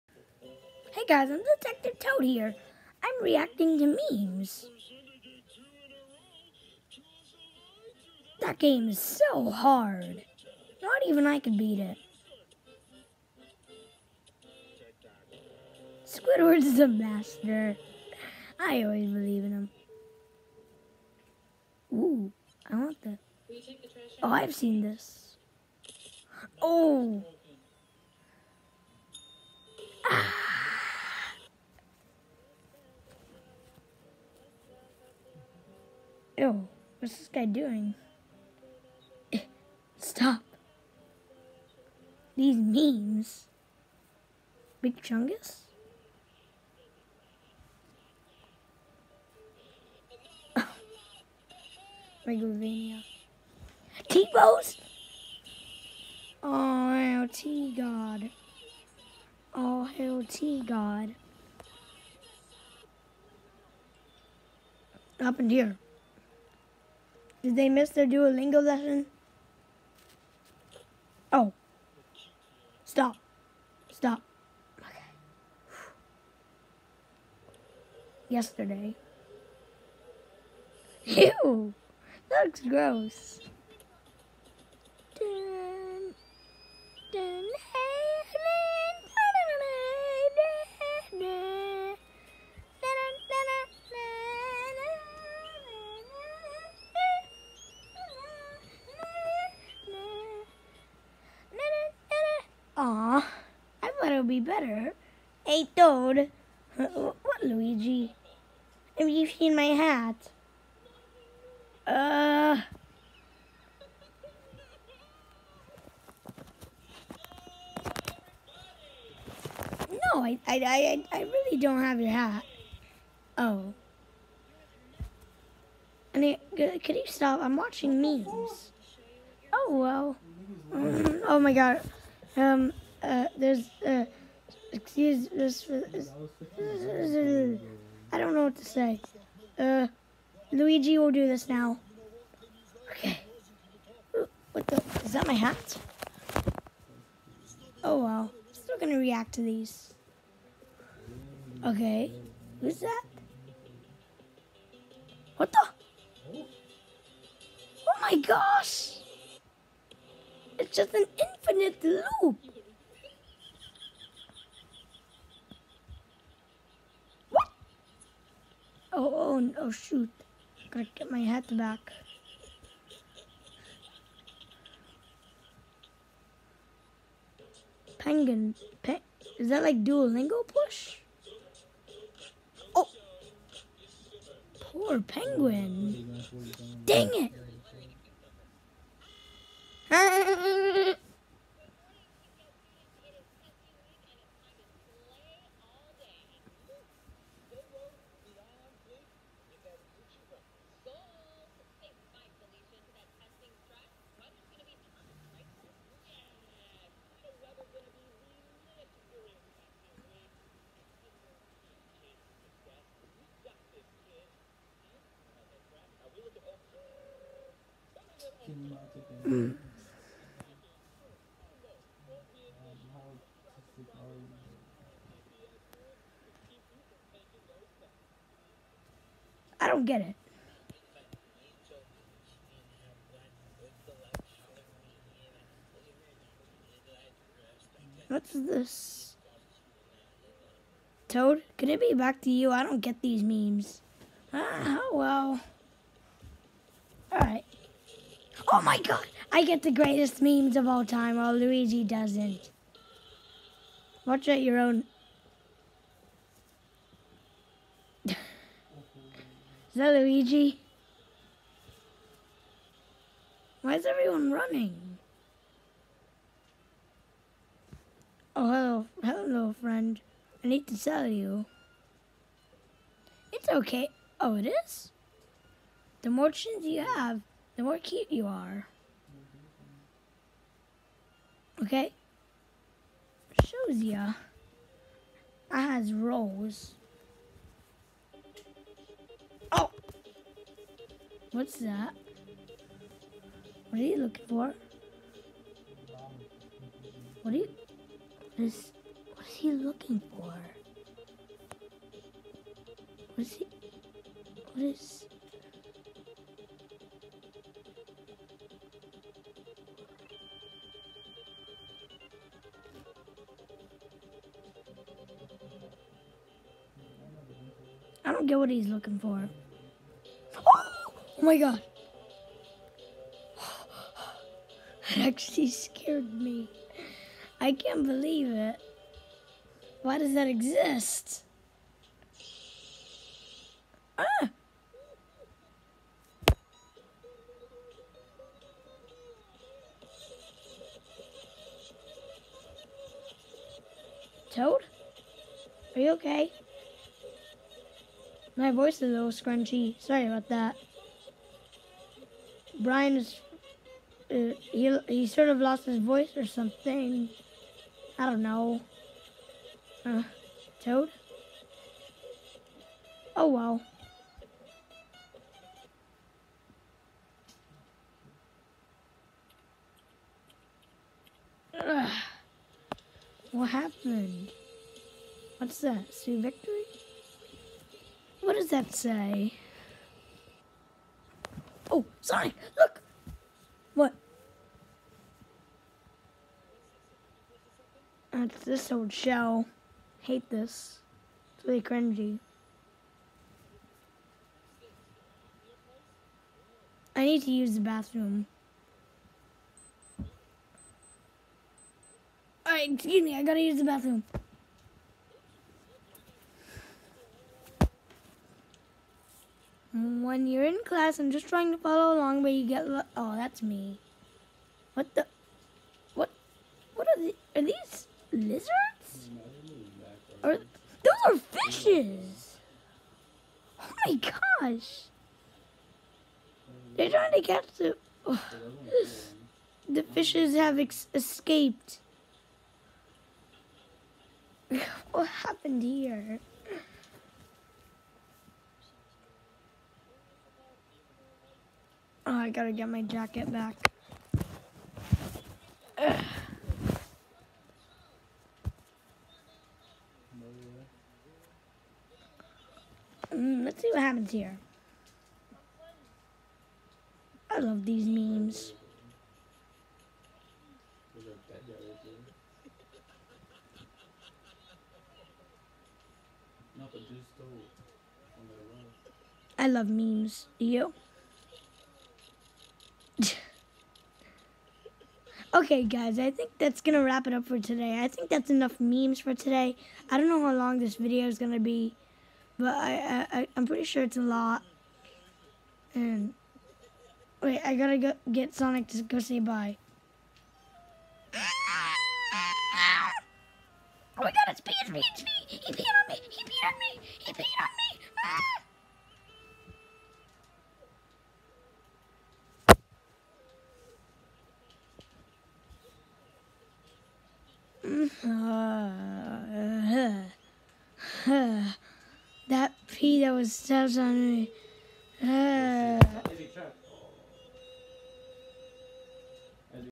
TOAD REACTS TO: MEMES
this episode detective toad is watching memes. Detective Toad is a new character in the Rexotik Channel. He will be in future videos, along with his buddy’s Luigi and Sonic.